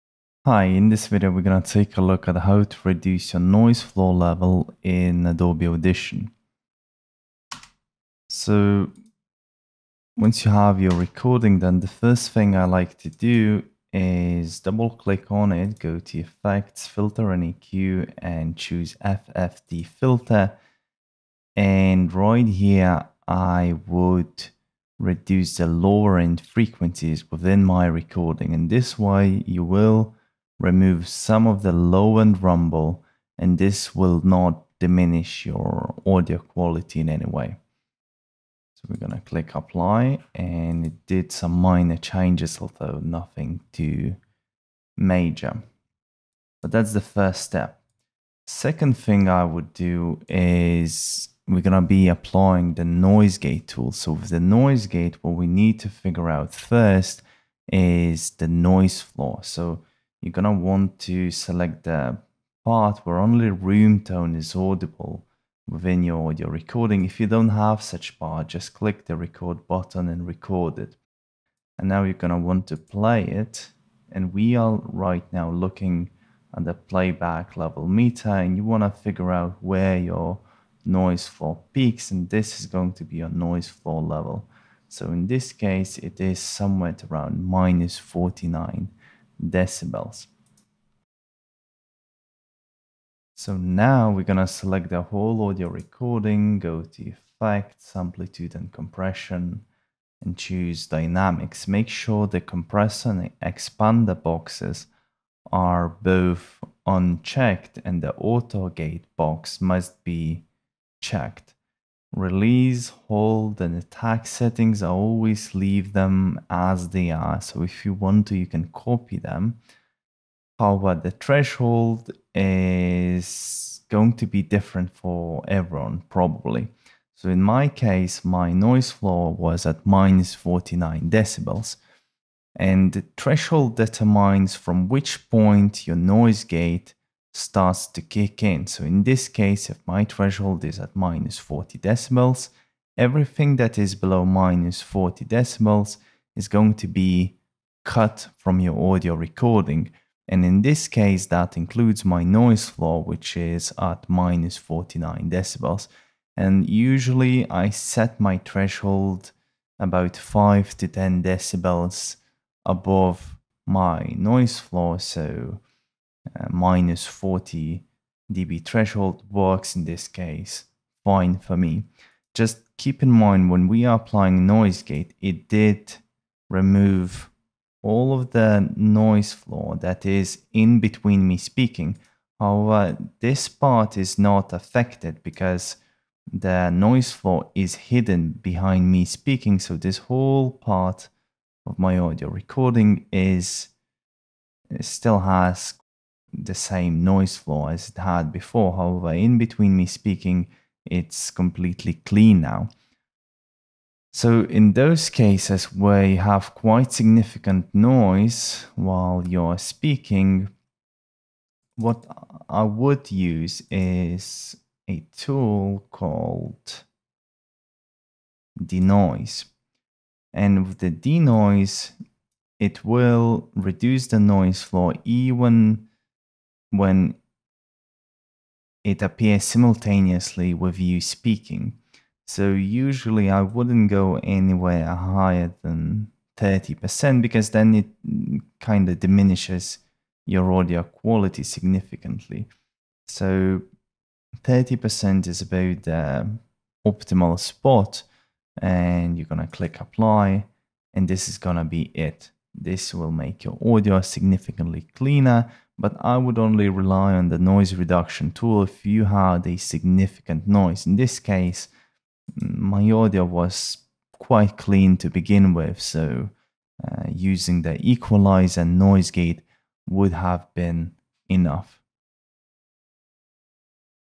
How to Reduce Noise Floor in Adobe Audition? | Tutorial
In this Adobe Audition tutorial, you'll learn how to measure and reduce or completely remove noise floor (background noise) from your audio recording within Adobe Audition's waveform editor using equalizer (EQ), noise gate and noise reduction (DeNoise) tools.

00:00 Equalizer
00:47 Measuring noise floor level
01:32 Applying noise gate
02:53 Noise floor behind vocals
03:23 DeNoise
04:15 When to use each effect